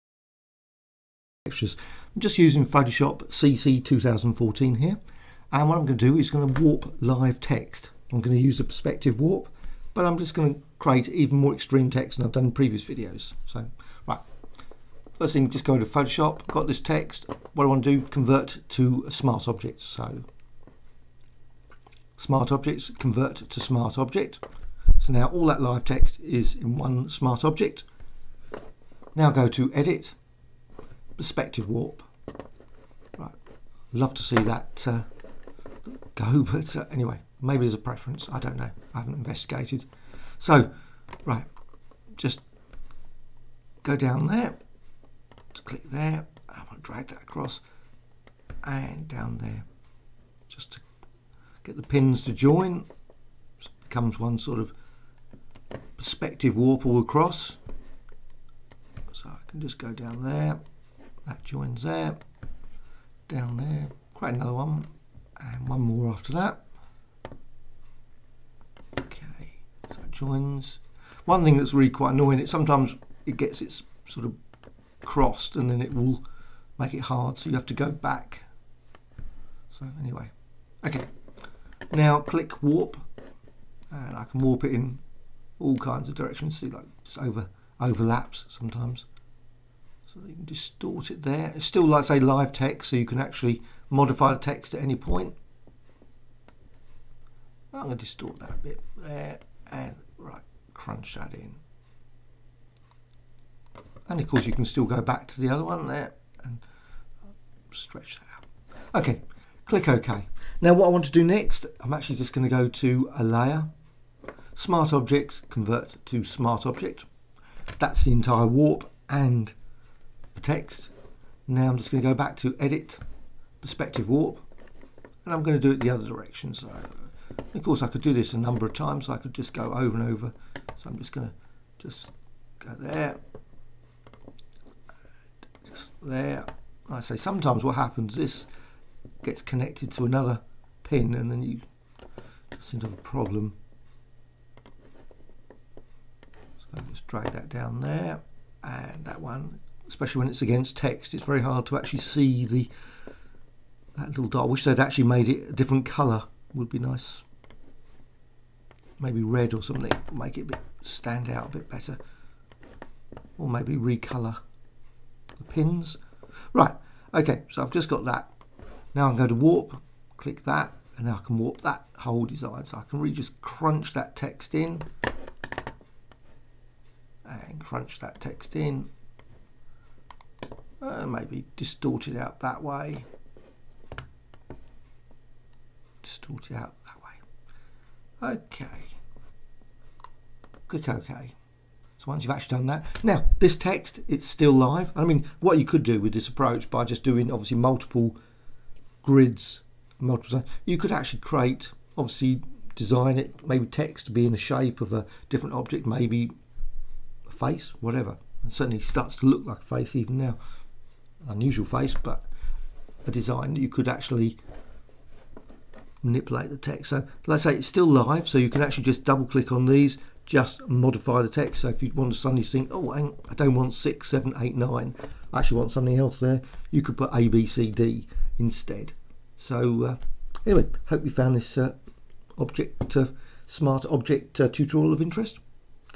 Photoshop CC -- Smart object Perspective warp grid and text tutorial
The perspective warp can be applied more than once to create extreme warping effects with live text in Photoshop CC. Apply once and then use a smart object and then perspective warp again

Photoshop CC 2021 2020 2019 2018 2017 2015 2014